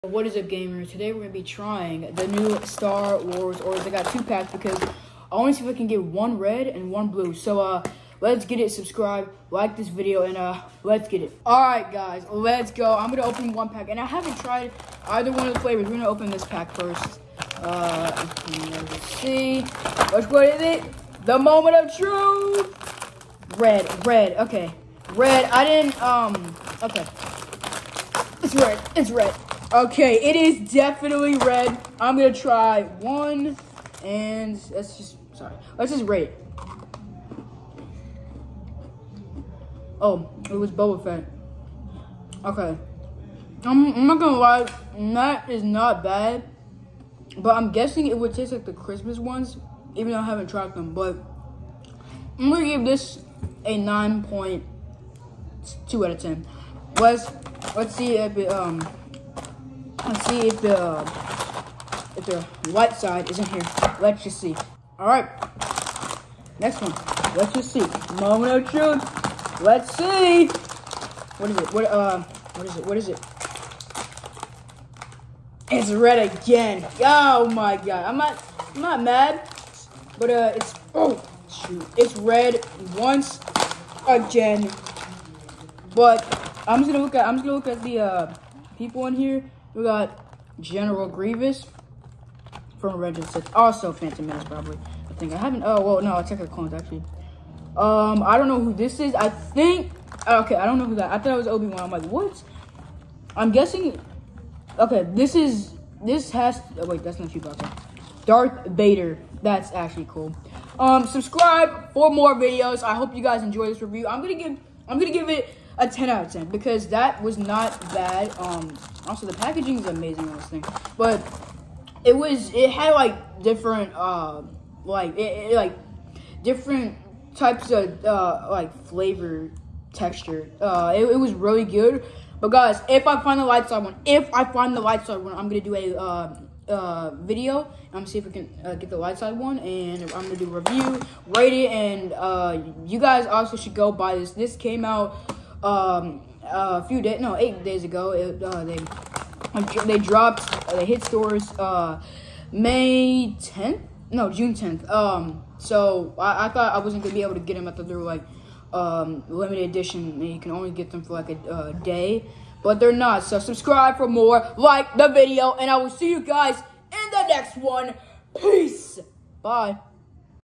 STAR WARS OREOS REVIEW!!!!!! WILL I GET LIGHT SIDE OR DARK SIDE
I FINNALY GOT MY HANDS ON THOSE STAR WARS OREOS I GOT 2 PACKS TO SEE IF I CAN GET BOTH LIGHT SIDE AND DARK SIDE!